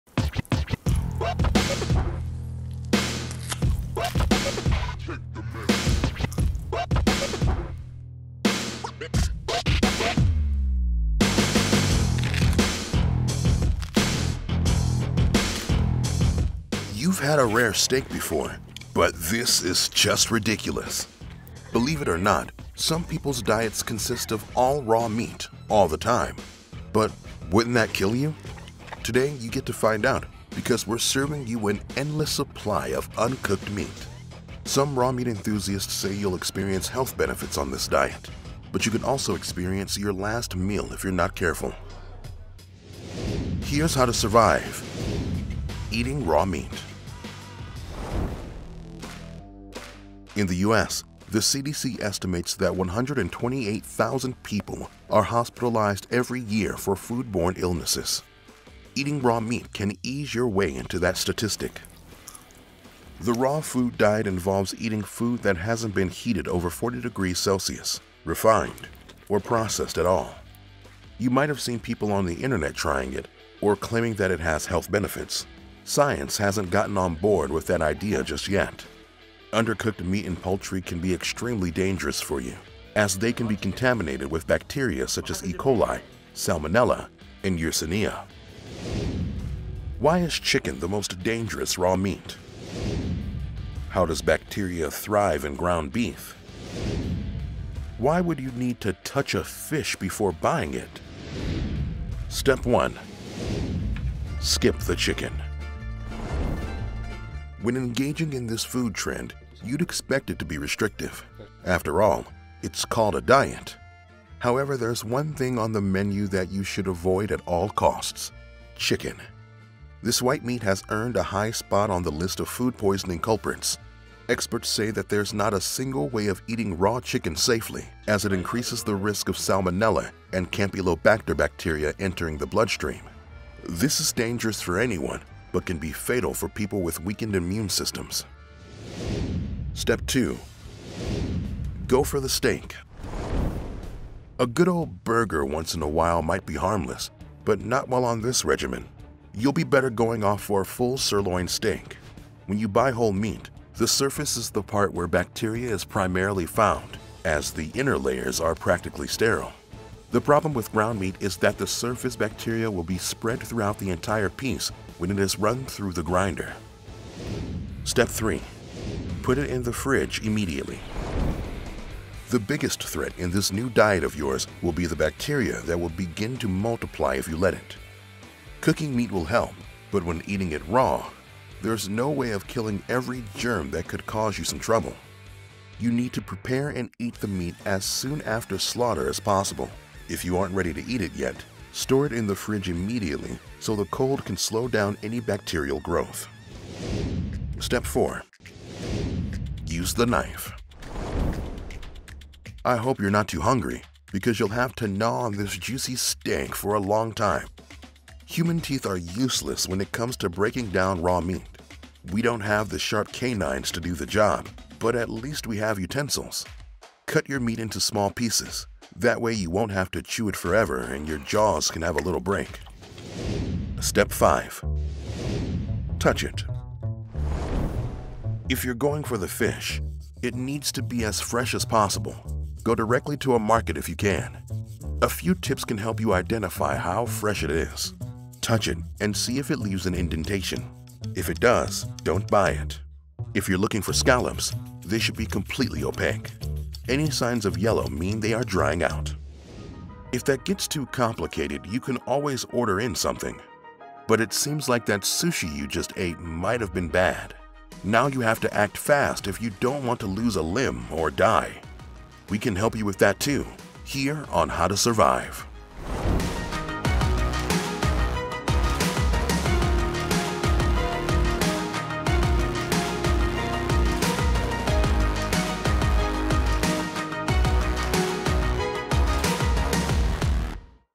How to Survive Eating Raw Meat
Keep away from the raw meat! This stuff is filled with bacteria, or is it? We've all heard of ceviche or tartare, but what other types of raw meat are edible? And what would a raw meat diet do to your body? These steps will help you survive your carnivorous journey.

01:41 1. Skip chicken
02:19 2. Eat steak
02:47 3. Refrigerate immediately
03:16 4. Use a knife
03:43 5. Touch it